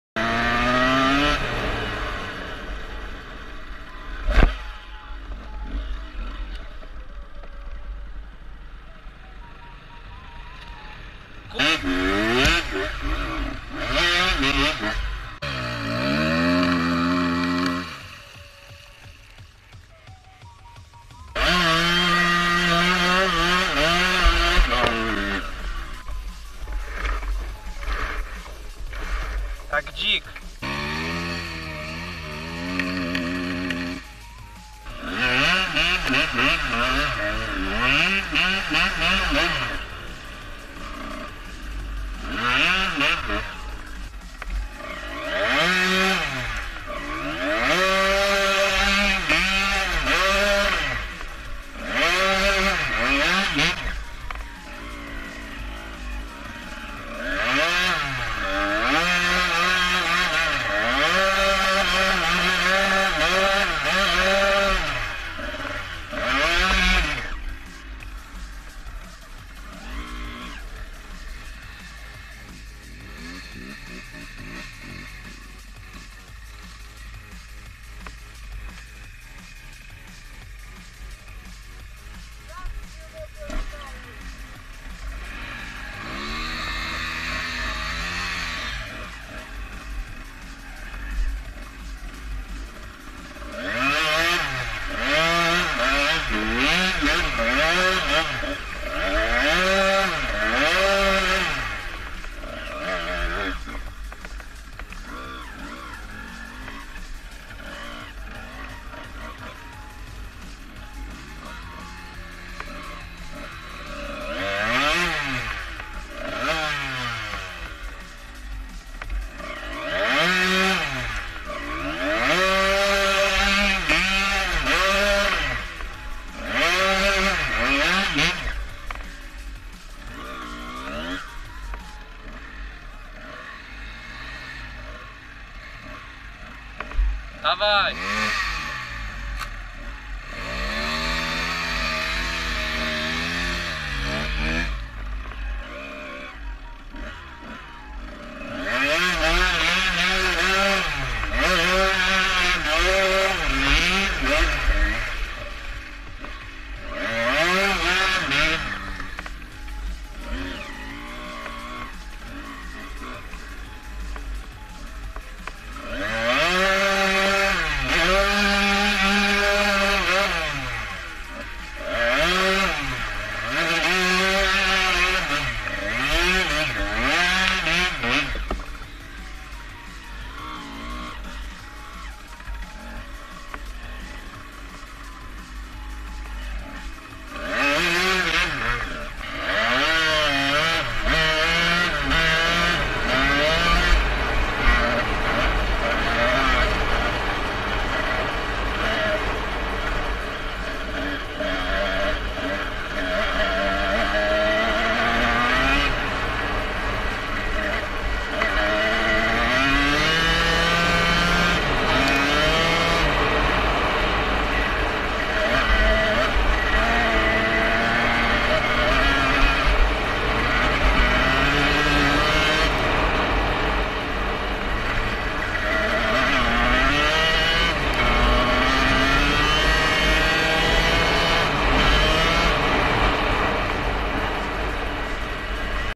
Jazda po błocie i wywrotka kolegi
Siema. Dziś odcinek z jazdy po błociei wywrotki kolegi. Zachęcam do zostawienia łapki w górę i skomentowania filmu. Do następnego Youuu.🔥🔥💪😈